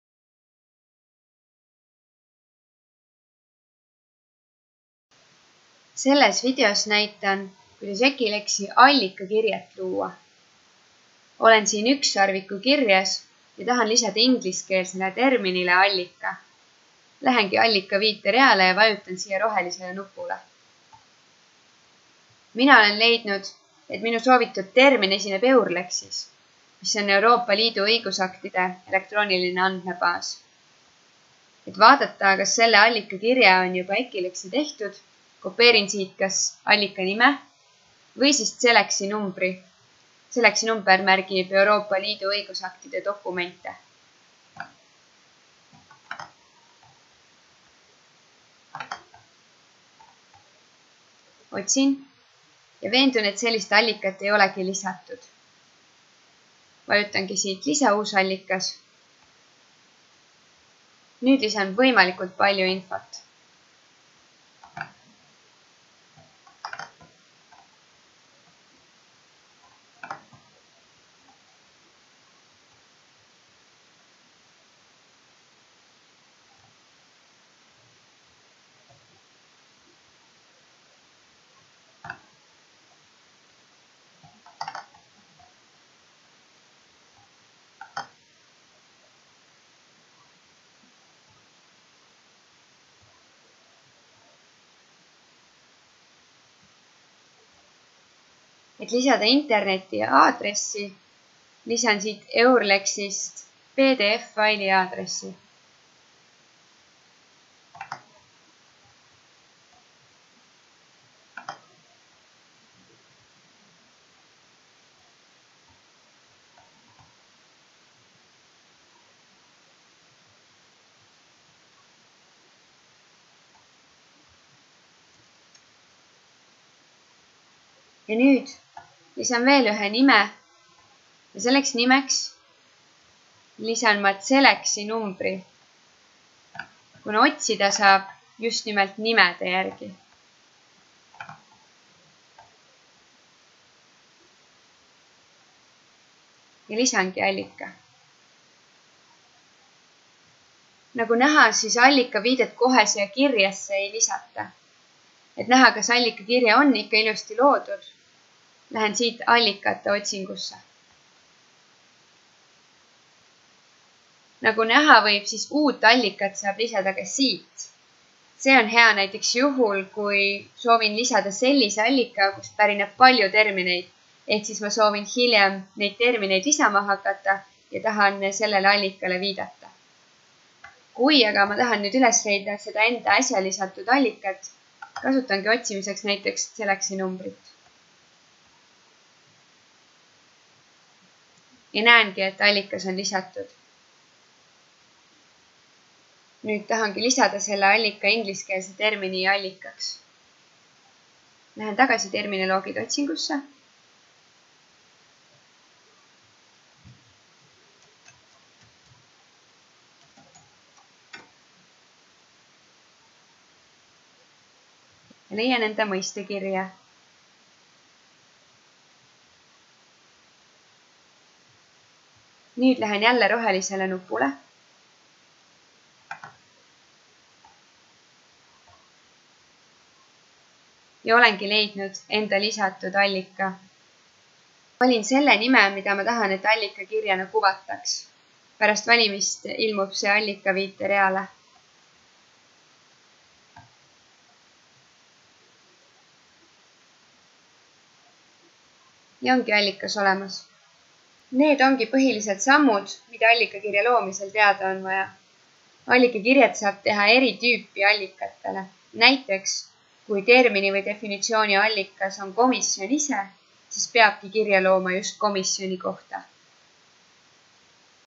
Allikakirje loomine Ekilexis
See video näitab, kuidas luua allikakirjet Eesti Keele Instituudi sõnastiku- ja terminibaasisüsteemis Ekilex